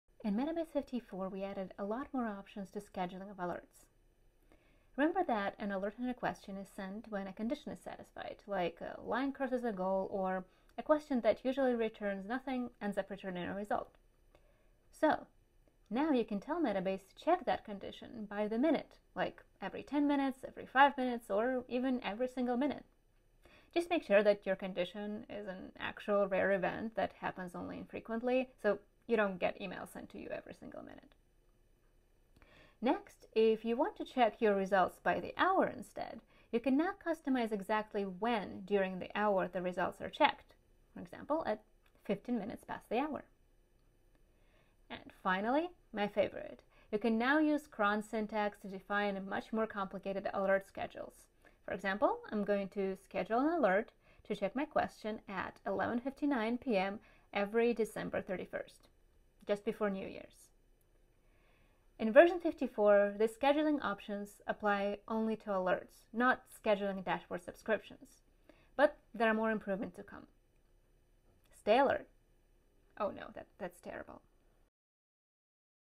New in Metabase v54: More control over alerts
Schedule alerts to repeat every few minutes, or at a chosen minute past the hour. For less frequent check-ins, schedule alerts monthly. And for complete control, use cron expressions to fine-tune your alert timing.